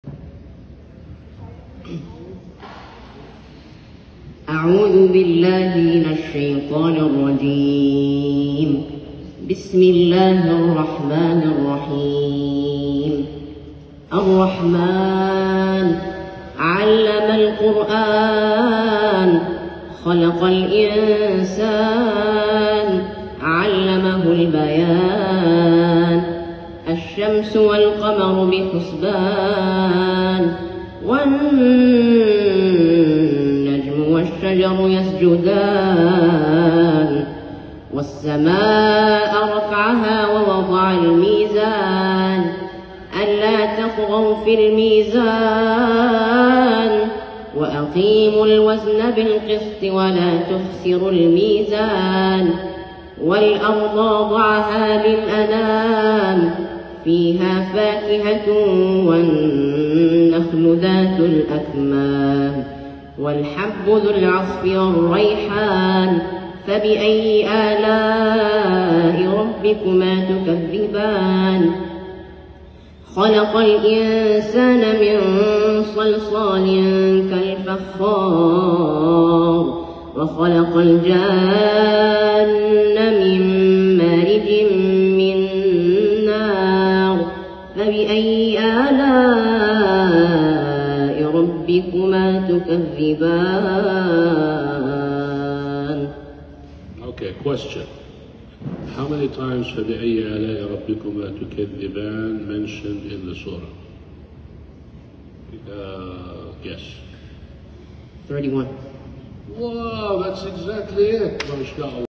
Awesome recitation by 8grade ISI student . Imam Hasan Khalil
Khalid Abdi an 8th grade Hafiz student.